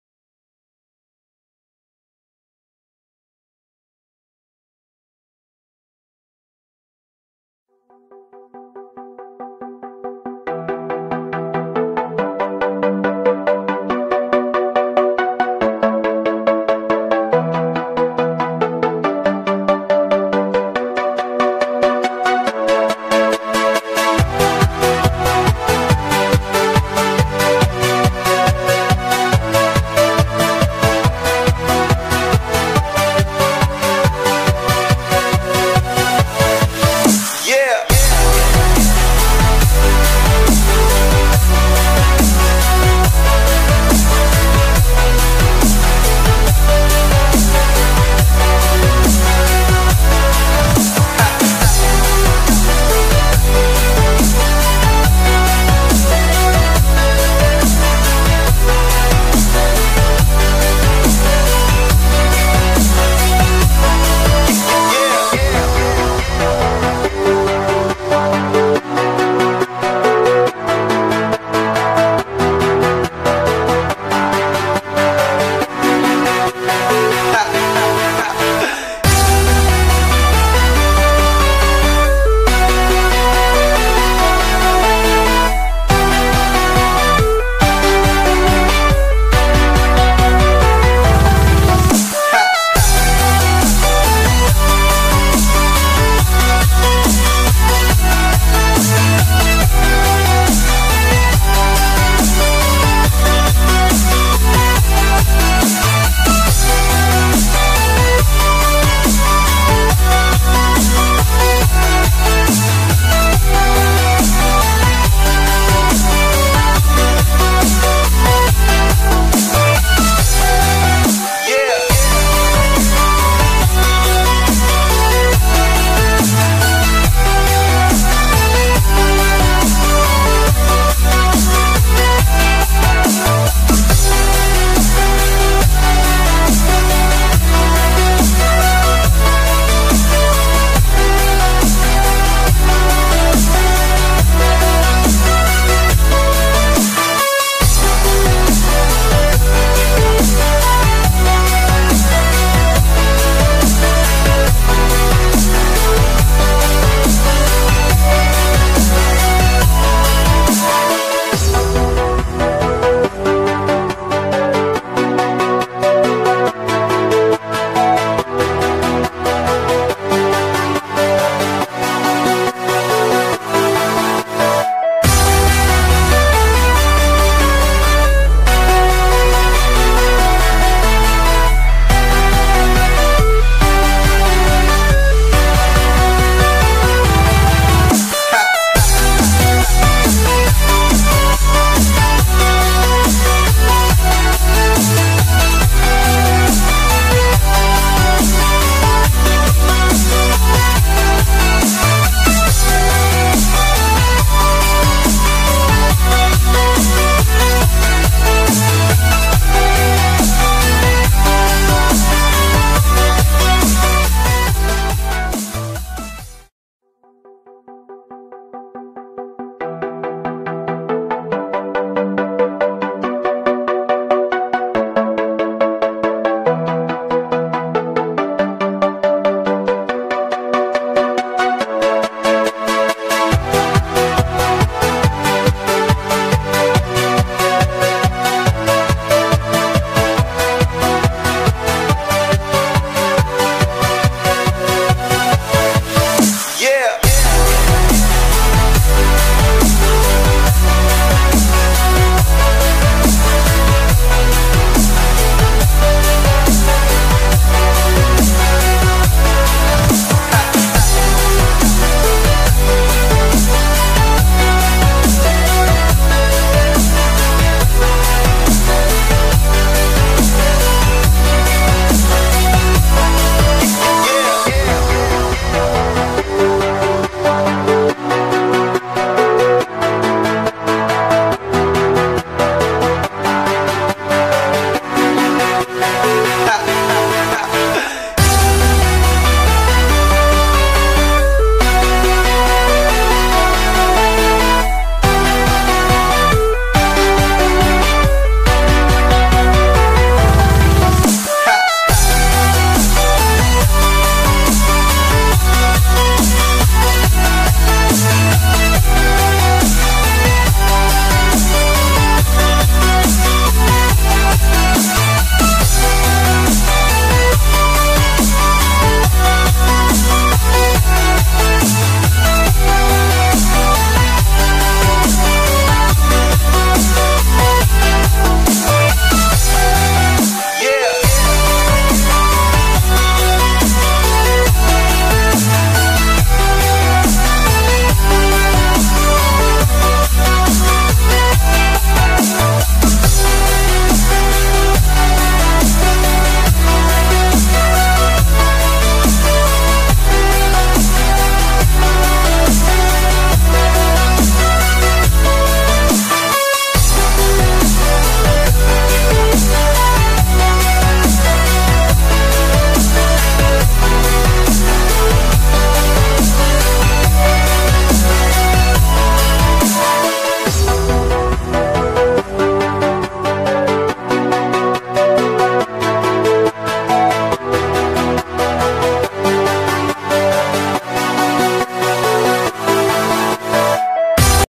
drone filma a noite show de weslei safadão no feste verão paraiba
show feste verão paraiba na praia de intermares